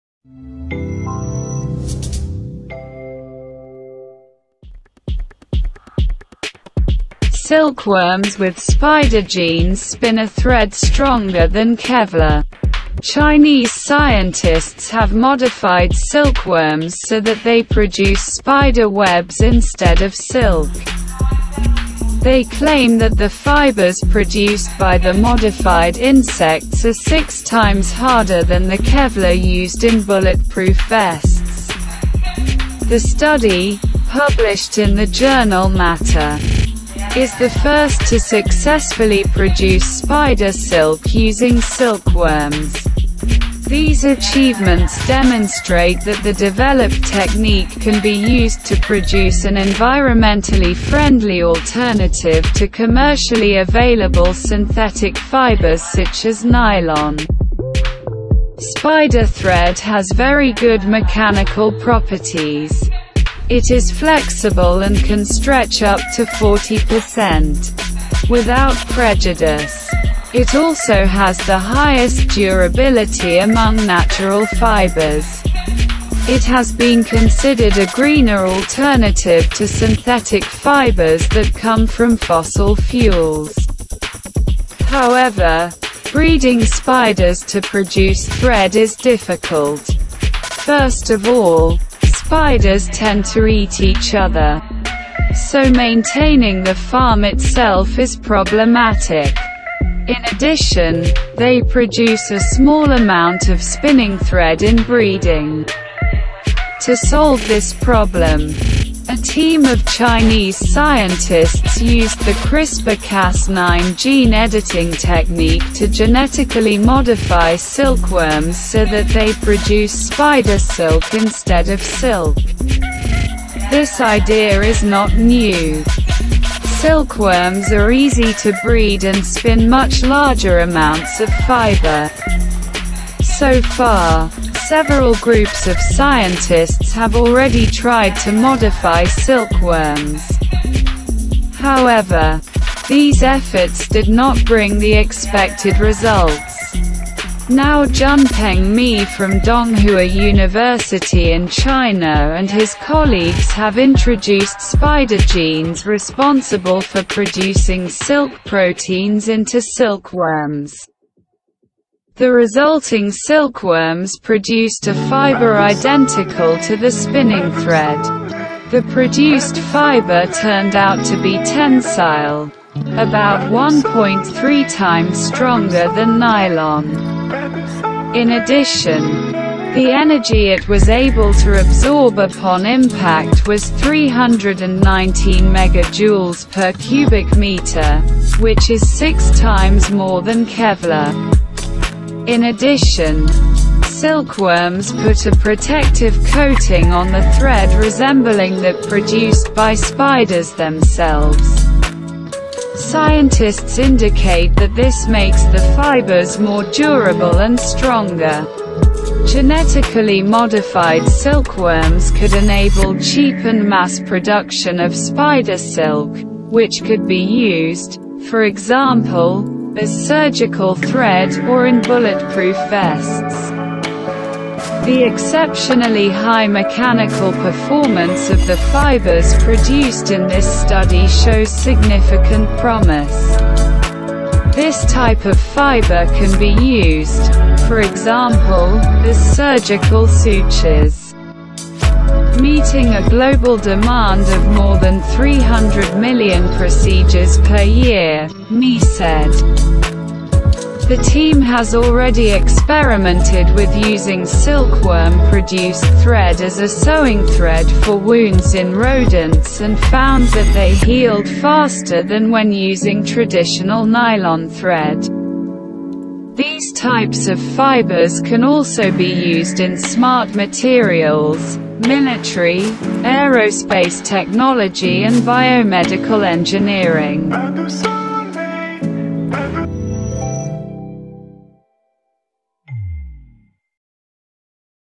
shoeplay vabTM
Topic:
Silkworms with spider genes spin a thread stronger than Kevlar.